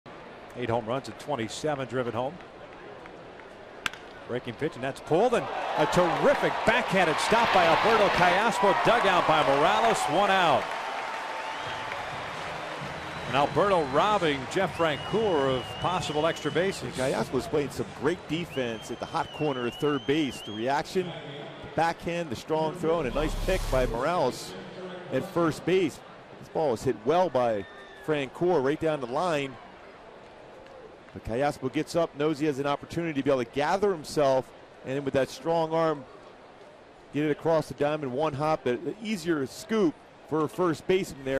2012/07/24 Callaspo's great stop
KC@LAA: Callaspo makes a great diving stop at third